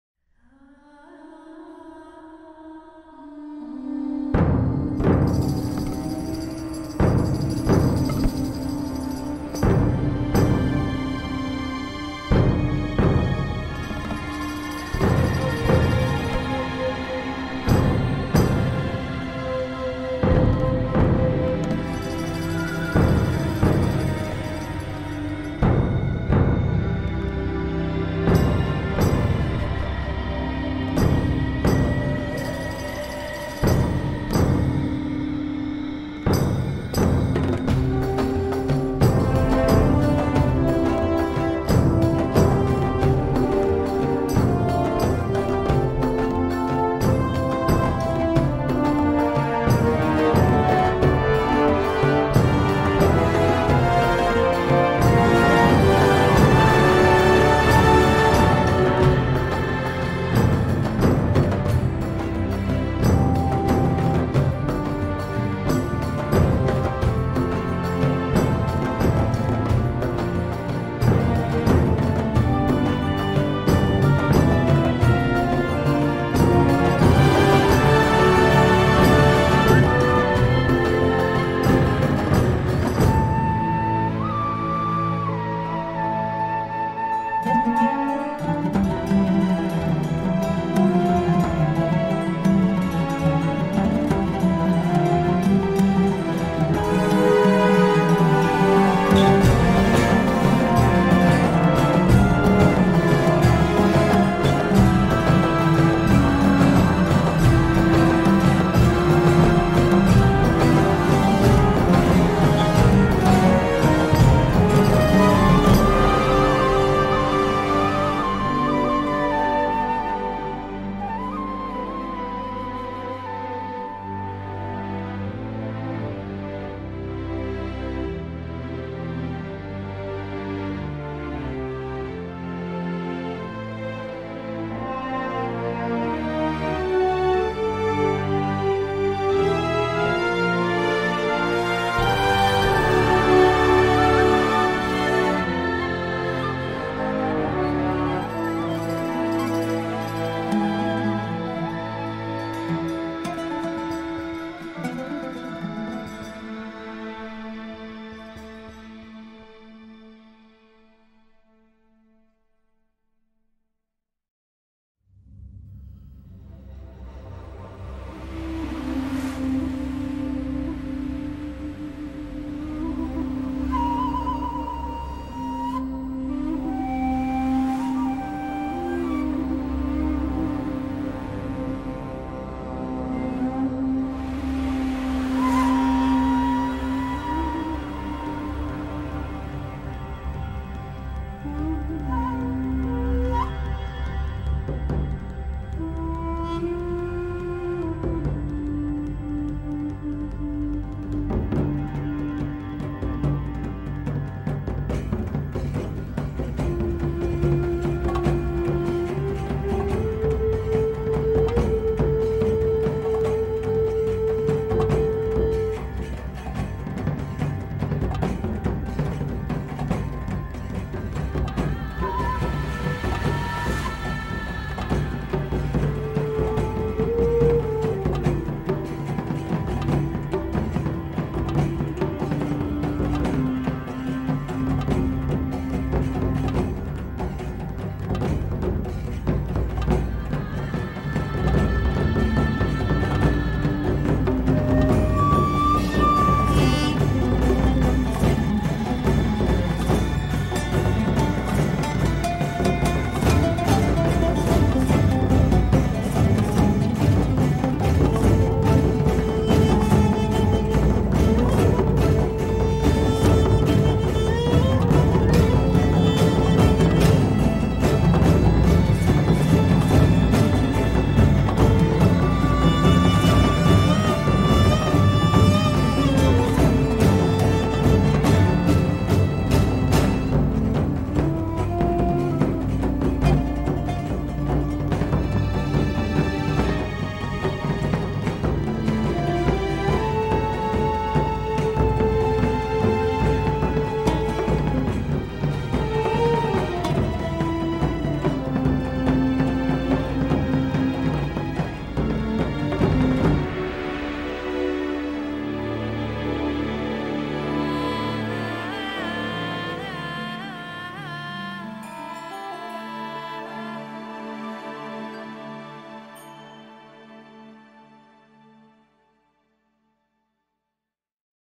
Desert Landscape - Speedpaint
Soft pastel painting of a dune, Im pretty proud with how it  turned out.